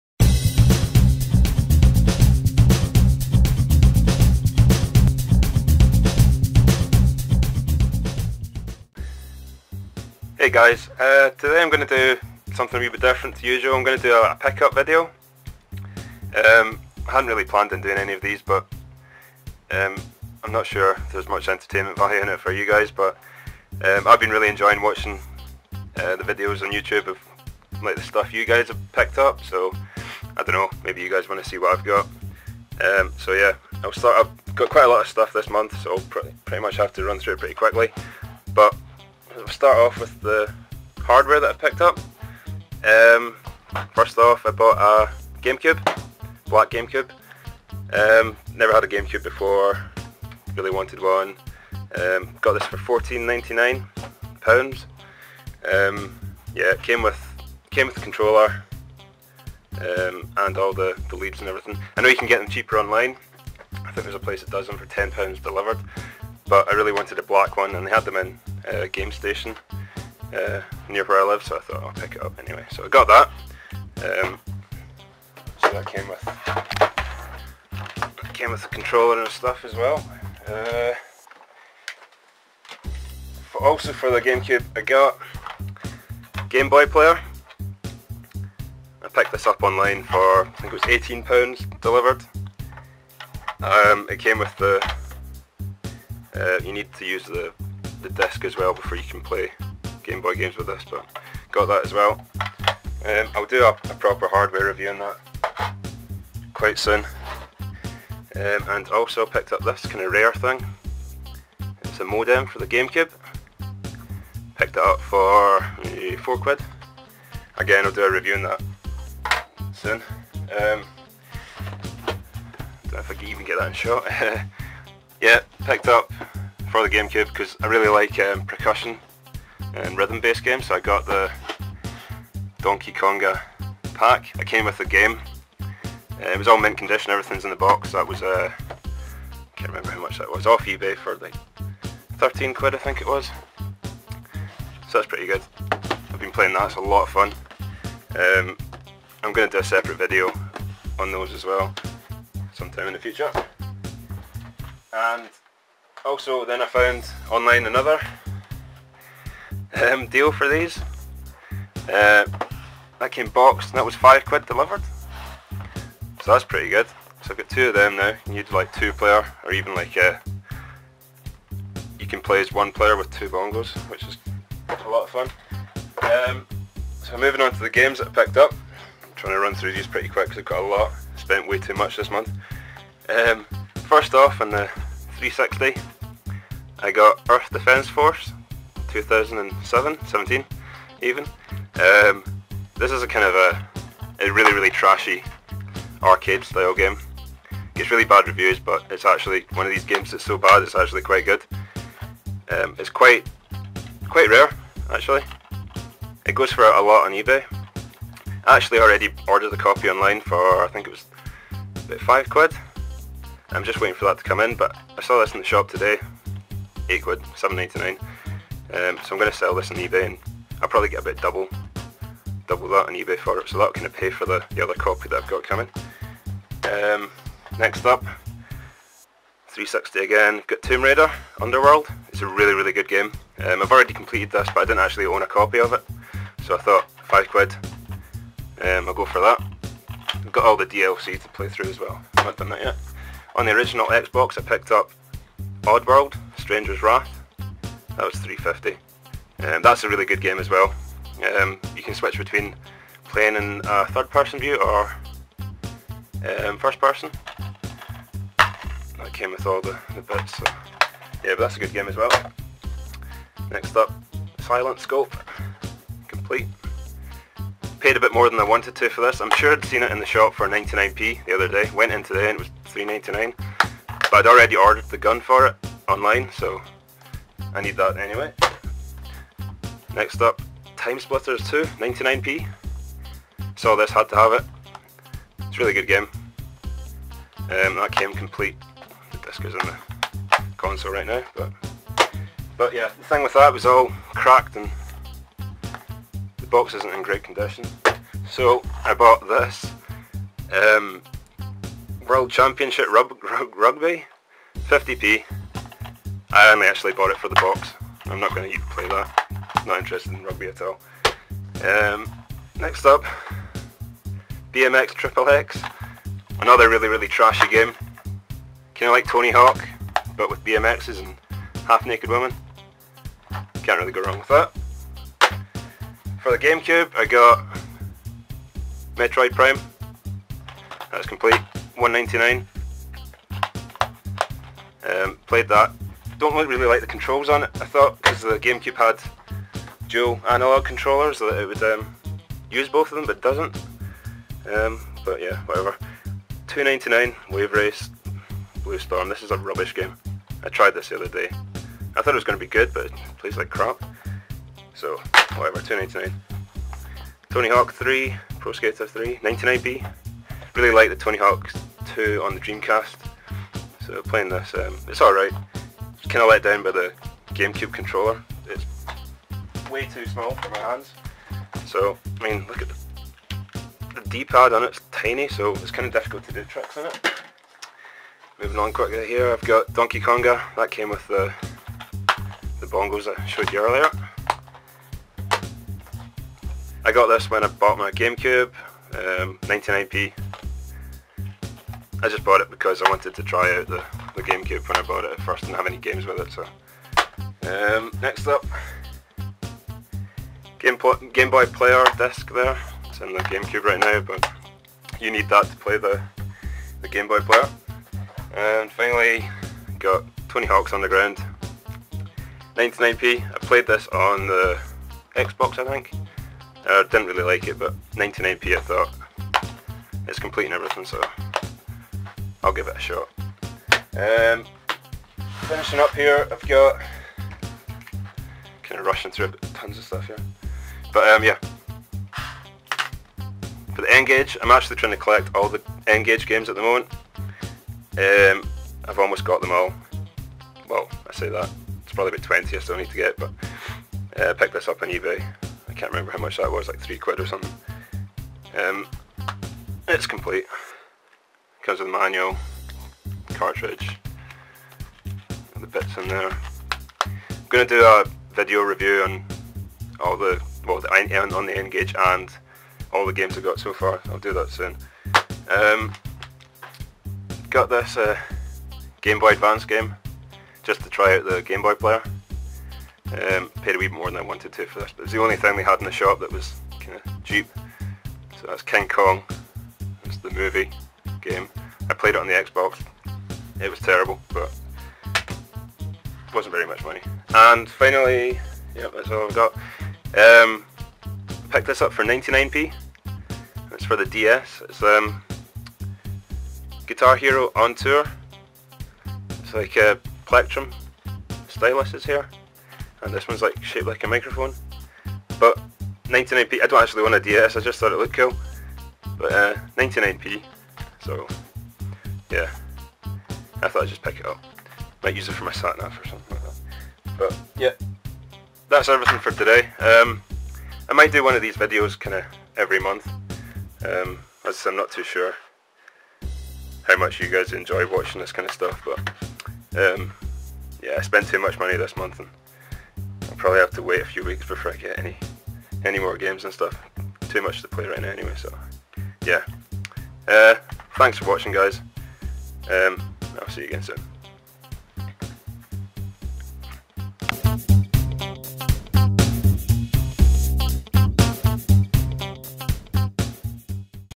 My Game And Hardware Pick Ups - Feb 2010
I've been really enjoying watching other peoples random gaming/console pick up vids here on YouTube so thought I'd have a go at doing one of my own. I picked up a load of stuff last month and at the start of this one so it seemed like as good a time as any to make my one. I like going round secondhand stores/charity shops/car boot sales etc and seeing what random bargains I can find. This month I found some cool hardware like the Gamecube modem and Bongos and some slightly obscure and trashy games such as BMX XXX and Earth Defence Force 2017.